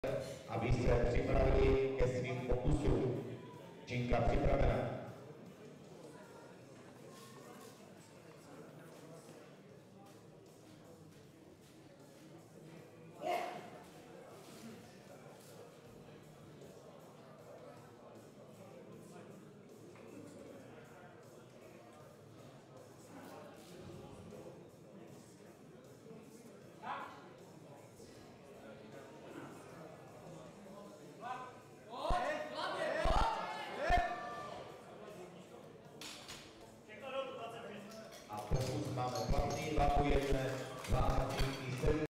WOLFOVÁ Lenka (1995), 3. pokus v benčpresu 70 kg
18. ročník Bohumínského benchpressu

Místo: Tělocvična TJ Viktorie Bohumín, Starý Bohumín